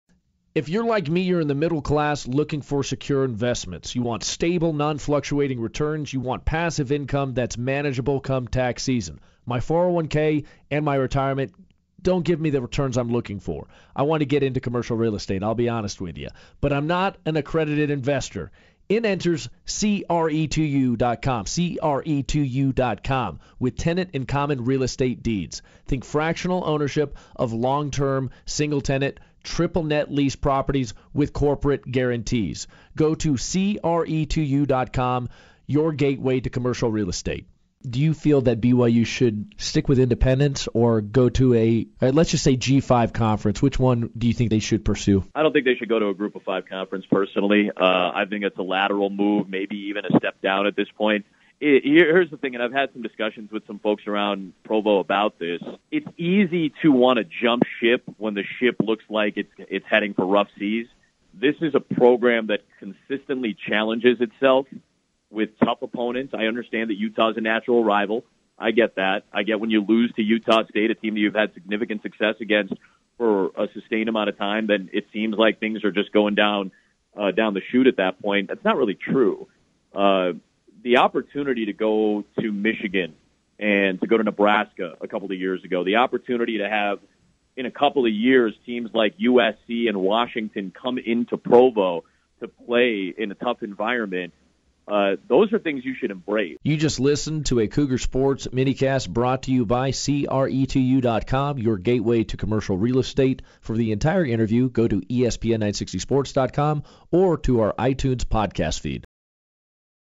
ESPN 960 Minicast: ESPN's Adam Amin talks BYU Football Independence
ESPN Play-by-Play Announcer Adam Amin joined Ben Criddle & Mitch Harper from ESPN 960 Radio in Utah to discuss BYU Football Independence.